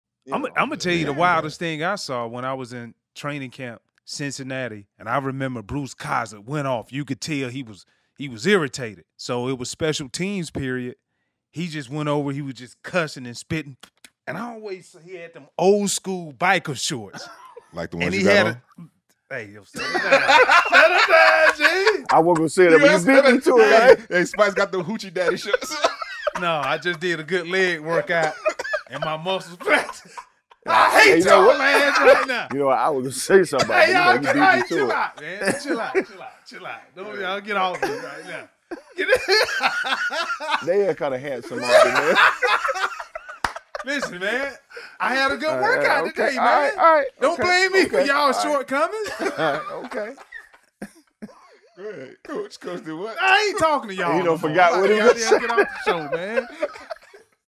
TAKEO SPIKES SPORTS HOOCHIE DADDY SHORTS | TUTAN REYES & DOMINIQUE WILKINS 🤣😂
Tutan & Dominique Wilkins have some fun at the expense of Takeo right in the middle of the interview. 😂🤣

Co-Host & Executive Producer: Tutan Reyes
Edited and YouTube Management by Devin Dismang